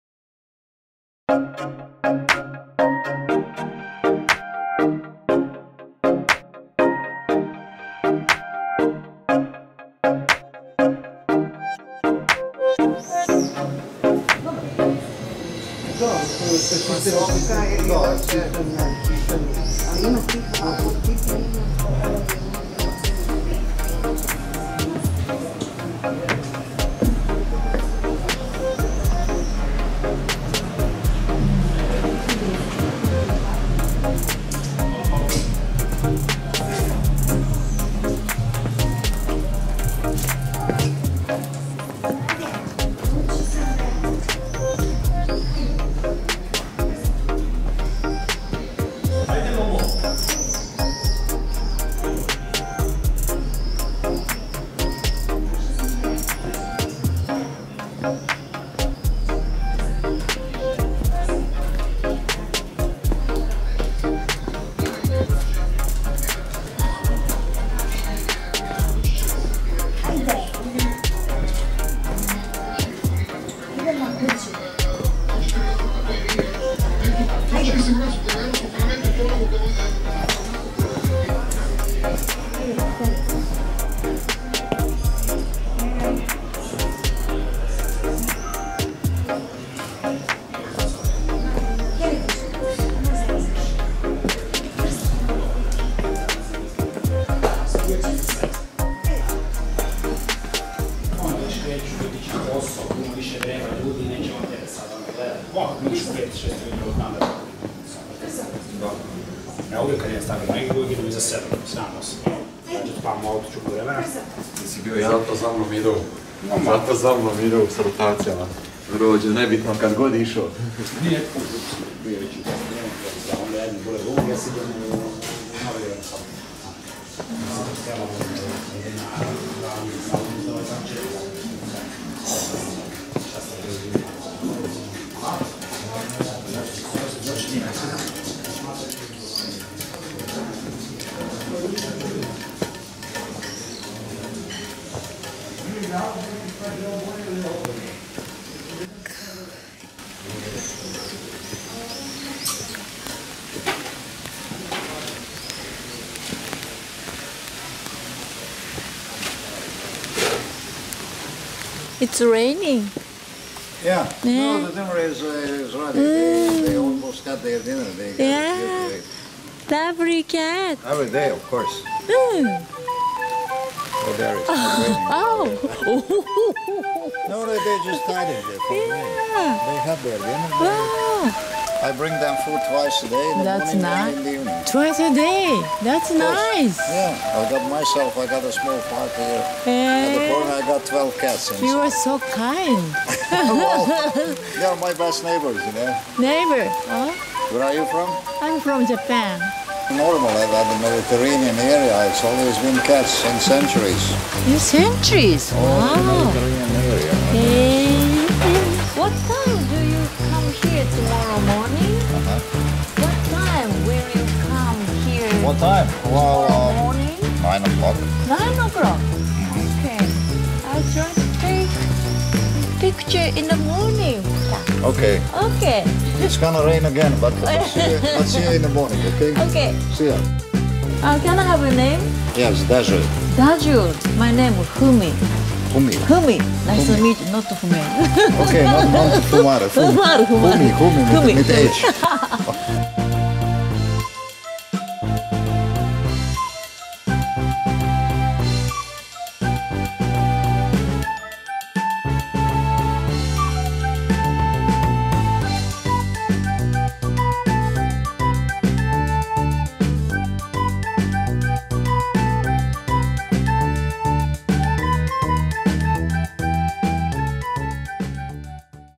旅ドキュメント第１話「クロアチアの猫好きおじさんに出会った」
石畳と細い路地が魅力のロヴィニ 。旧市街は世界遺産です。
夕景を見に教会まで散歩していたら、猫好きおじさんに出会ってしまいました。
1日2回えさやりをしているというダジュールさん。
明日の朝9時に、またここで会うことになりました。
シリーズ第１話です。
続きは、もうすぐ作りますんで！笑

見ていただいて有り難うございます！